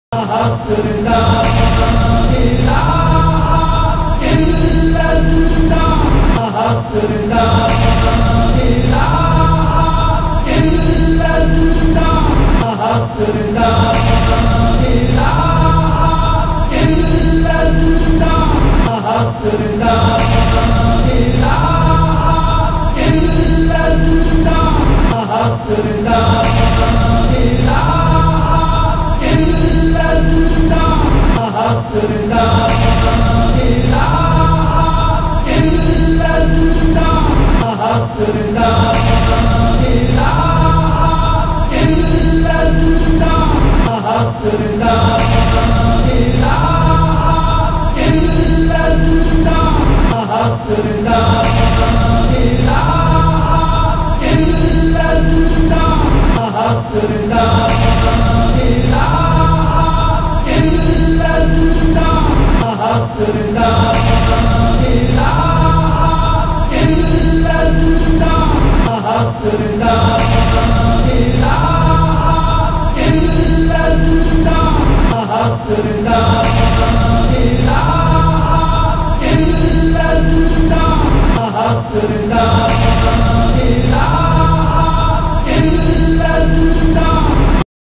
LA ILA HA ILLALLAH DHIKR📿💎📜
This is a best zikr to remove black spot from the wall of heart. When a person did bad in his/her own life then he/she get black spot on the their own heart. this Dhikr/zikr remove that black spot.